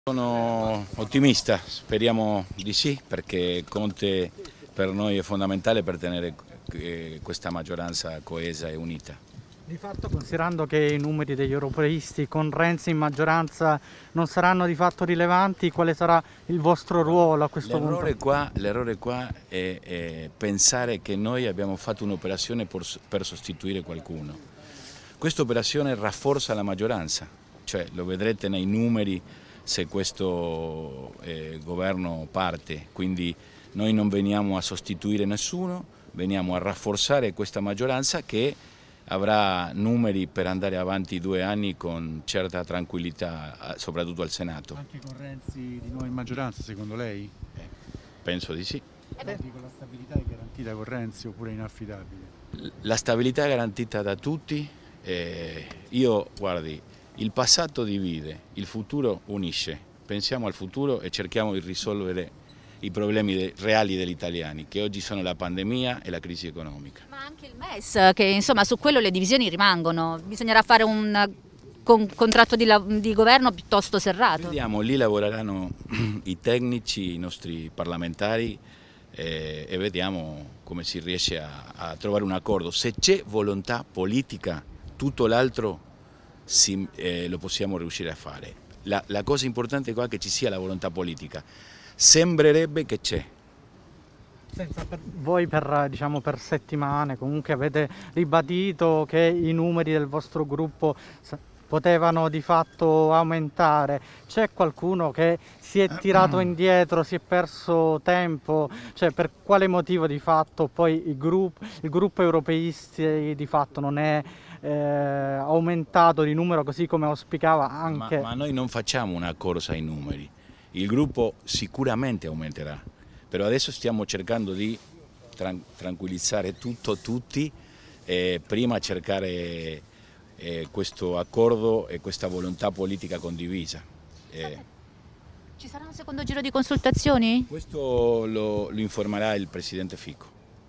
Consultazioni, Merlo: "Nostra operazione non sostituisce nessuno"
"Rafforziamo una maggioranza che avrà numeri per andare avanti due anni"

POLITICA (Roma). "Sono ottimista. Per noi Conte è fondamentale per tenere questa maggioranza. Non abbiamo fatto un'operazione per sostituire qualcuno. Questa operazione rafforza la maggioranza. Non non veniamo a sostituire nessuno ma a rafforzare una maggioranza che avrà numeri per andare avanti due anni con tranquillità. Anche con Renzi? Penso di sì. Il gruppo sicuramente aumenterà". Lo ha detto Ricardo Antonio Merlo, del gruppo Maie-Europeisti al Senato, al termine delle consultazioni con il presidente della Camera Roberto Fico. (Andrea Corti/alanews)